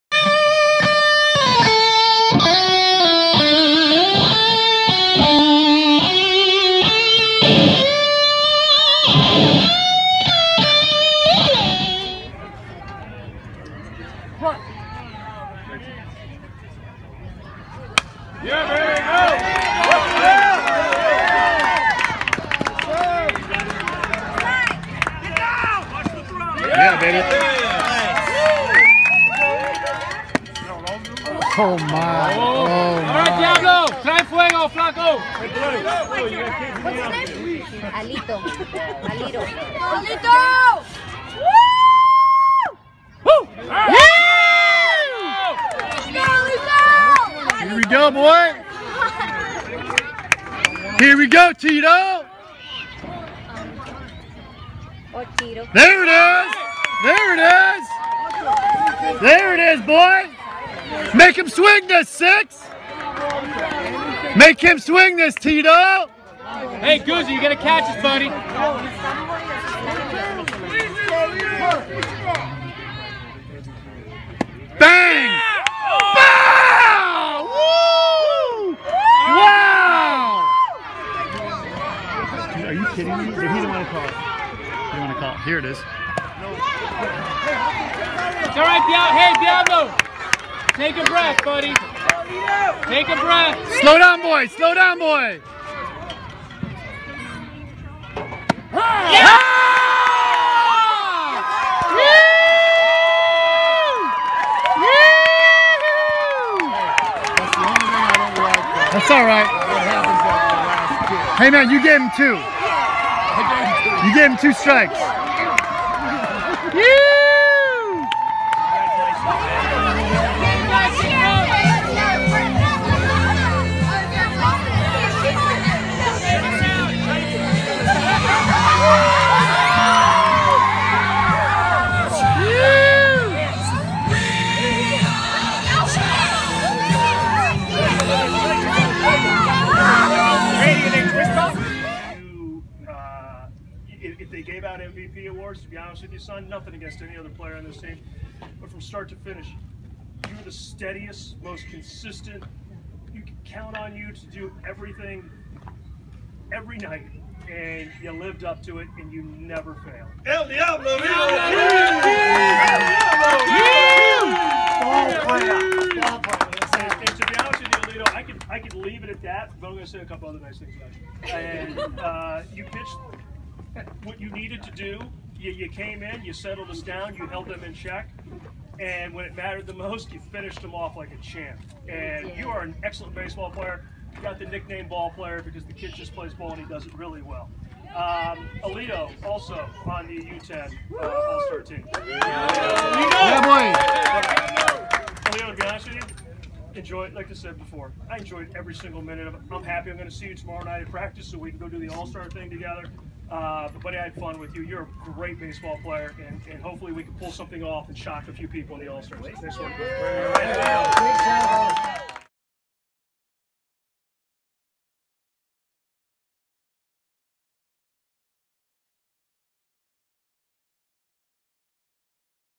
Team Spyder...Hb Champs 2016
Alito pitches the Spyder AAA bball team through the losers bracket to clinch the championship 2016